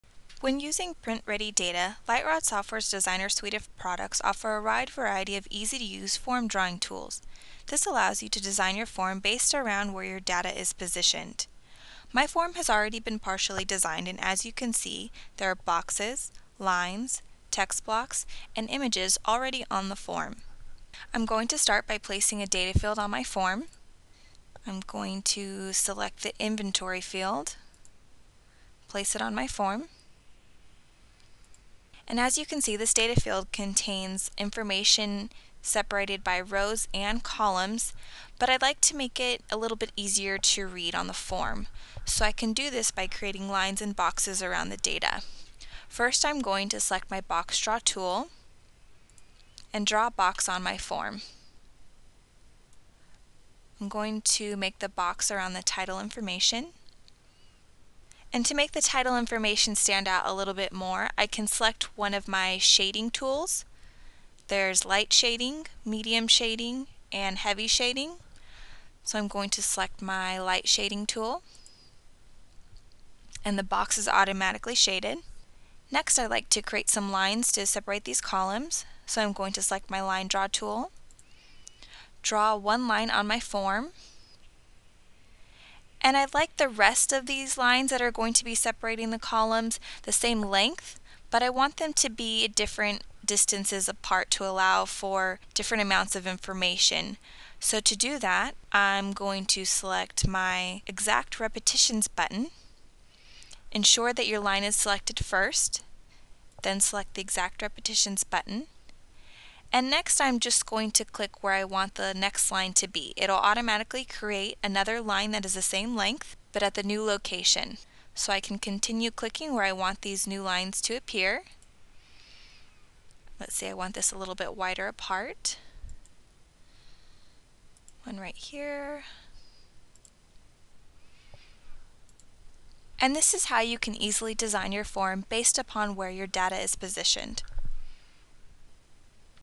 Invoice - Basic Form Draw tools
Lytrod Software offers a wide variety of form drawing tools. Learn how easily Lines, Boxes, Shading, and more were easily added to any form on top of existing print ready data.